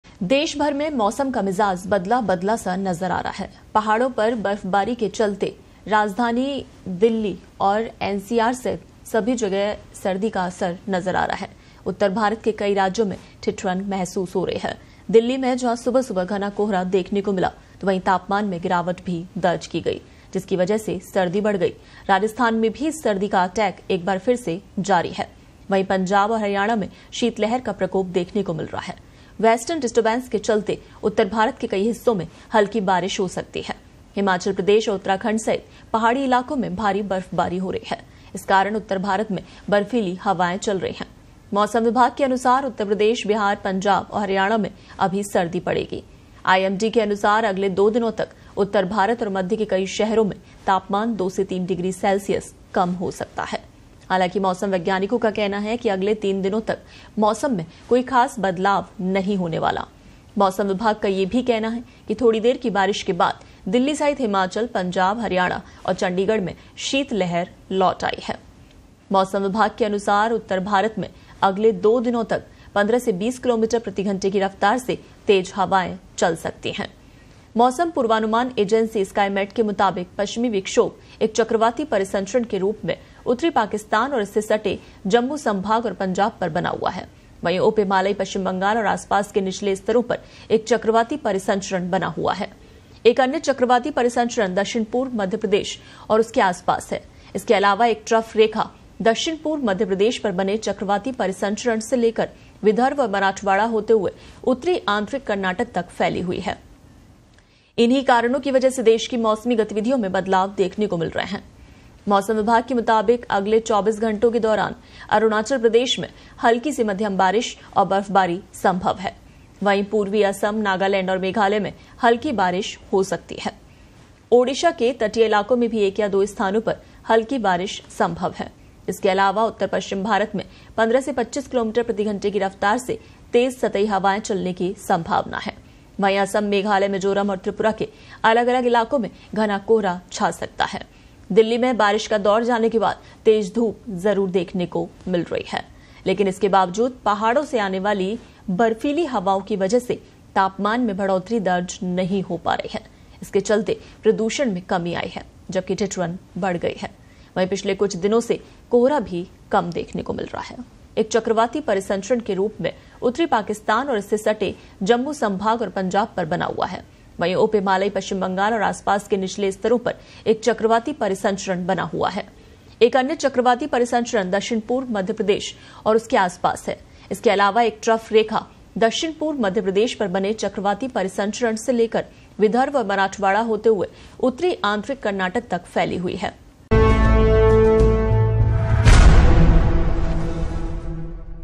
Weather Update Today: यहां बारिश-ओले की चेतावनी, IMD का अलर्ट | Weather Latest News
Weather Update Today: यहां बारिश-ओले की चेतावनी , IMD का अलर्ट  | Weather Latest News | Today Mausam Update | Breaking News | Mosam ka hal

देशभर में मौसम का मिजाज (Weather Update Today) बदलने लगा है..पहाड़ो पर बर्फबारी के चलते राजधानी दिल्ली सहित उत्तर भारत के कई राज्यों (Weather Latest News)में ठिठुरन महसूस हो रही है...इसी बीच मौसम विभाग (IMD)ने बड़ा अलर्ट जारी किया है. उत्तर पश्चिम और पूर्वी भारत में बारिश का दौर जारी रहने की संभावना है...जिसके बाद अगले 4 से 5 दिनों तक मौसम शुष्क रहने का अनुमान है...सबसे पहले बात दिल्ली (Delhi Weather Update) की करें तो आज सुबह कोहरा देखने को मिला...11 फरवरी(Today Mausam Update) तक दिन का न्यूनतम तापमान 7 से 8 डिग्री रहने का अनुमान है.. जबकि अधिकतम तापमान 22 डिग्री सेल्सियस रहने की संभावना है...मौसम विभाग के अनुसार हिमाचल प्रदेश, सिक्किम, अरुणाचल प्रदेश में हल्की से मध्यम बारिश और बर्फबारी की उम्मीद है...इसके अलावा पूर्वी उत्तर प्रदेश, बिहार (Bihar Weather Update) , झारखंड और पश्चिम बंगाल के कुछ हिस्सों में सर्दी बढ़ने की संभावना है.